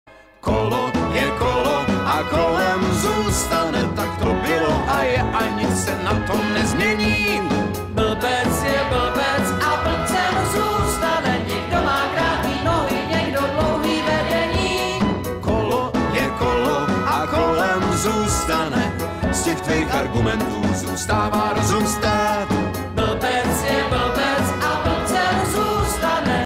honem na letiště
u kterého hotelu jezdí A6tky?
BTW: asi bych v tom sedět, jako zákazník, nechtěl :-(
hudba: The Tap Tap